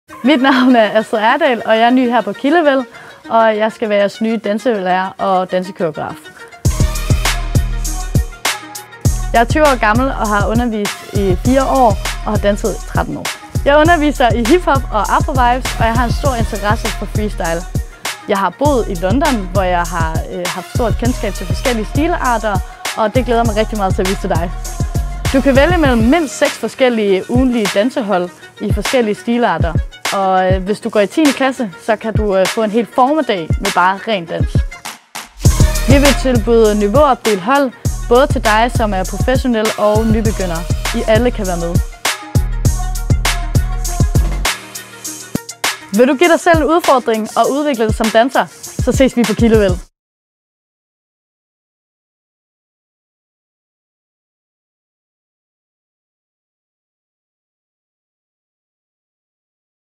Dans på Efterskolen Kildevæld
Skal du snart på efterskole? Og er du vil med dans? Så har vi stedet for dig! 🔥 På Efterskolen Kildevæld vil du opleve et seriøst dansemiljø, men dygtige dansere, fede instruktører og en masse opvisninger.

I år har vi ansat den dygtige Astrid Erdal, der er lig med fede moves og stor erfaring. Astrid har, på trods af hendes unge alder, undervist i mange år og danset i endnu flere.

Kom forbi Efterskolen Kildevæld til Efterskolernes dag den 26. september mellem 13 og 17 og mød hende - og oplev hvad hun kan! 🤩

Vi ses!